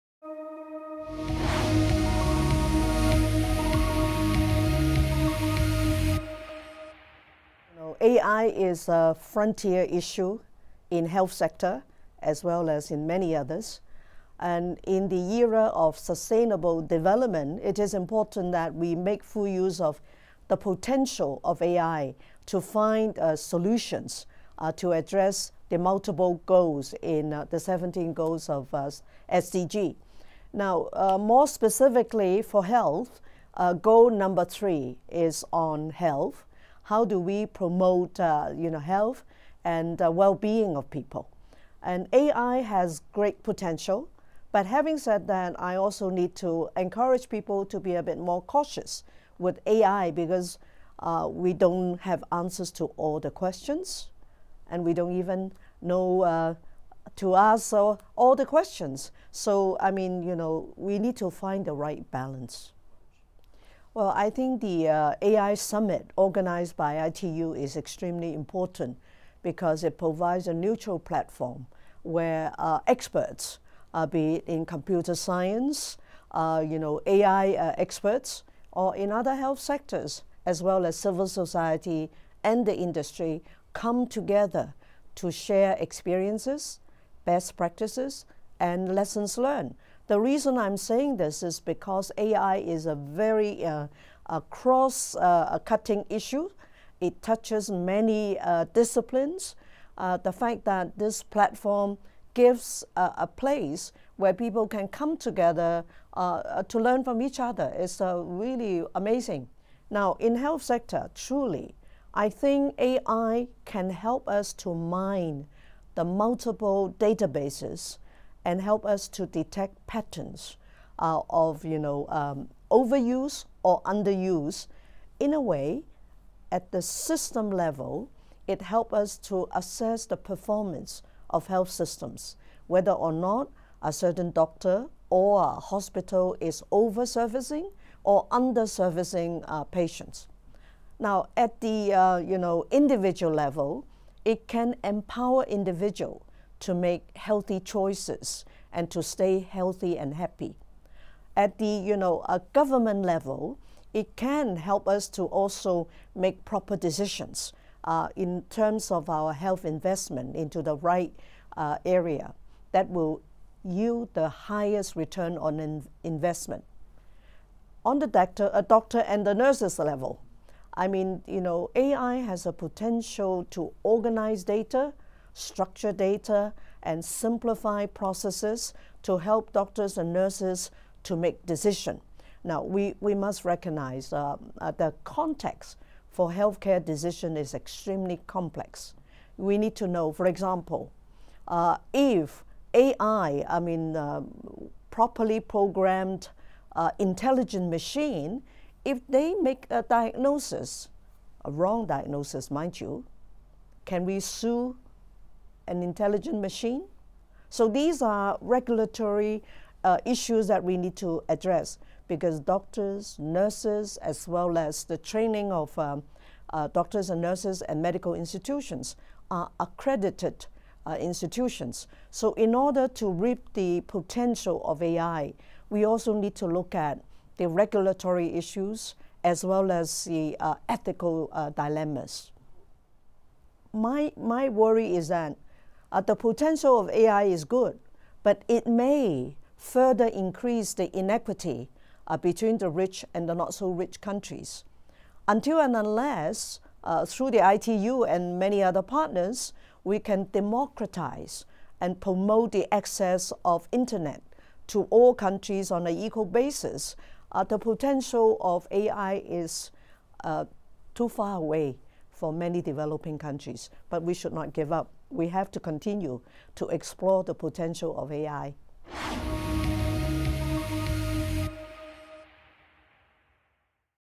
AI for GOOD INTERVIEWS: MARGARET CHAN, Director General, WHO
Interview with Dr Margaret Chan, Director General, World Health Organization at the AI for GOOD Global Summit, ITU, Geneva, Switzerland.